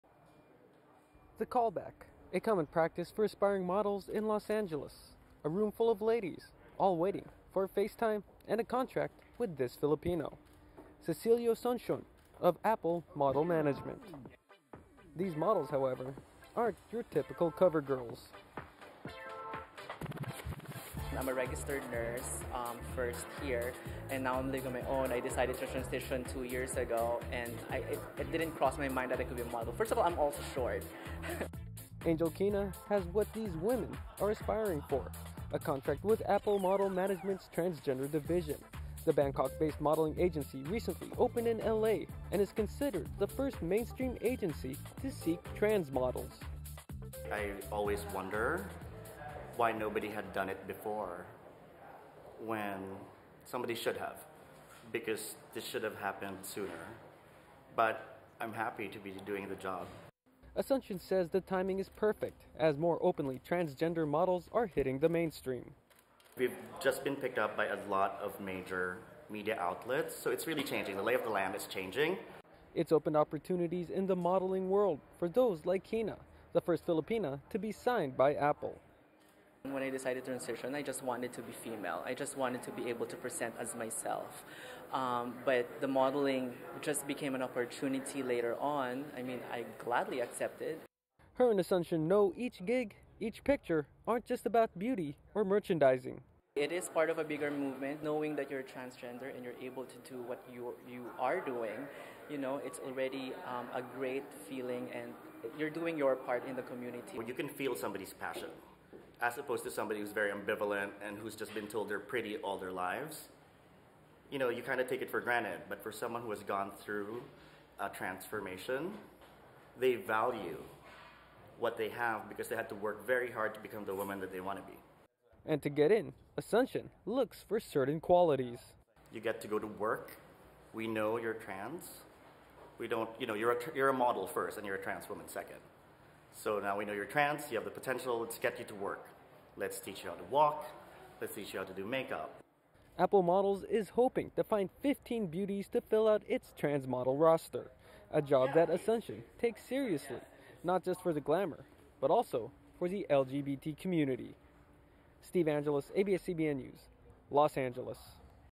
Fil-Am heads fashion industry's first transgender modeling agency
The world of fashion just got a little more diverse thanks to the launch of a new agency for transgender models. As Steve Angeles tells us, the search for trans talents is being led by a Filipino American.